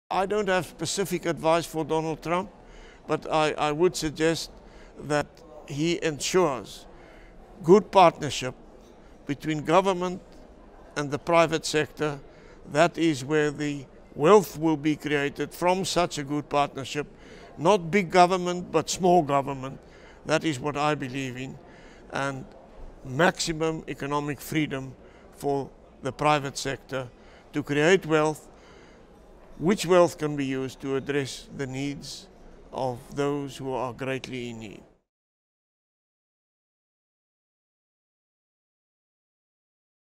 F.W. de Klerk shares his thoughts on Donald Trump
Gavin Serkin, Frontier Funds, interviews former President of South Africa and Nobel Peace Prize winner F.W. de Klerk at SuperReturn Africa in Cape Town.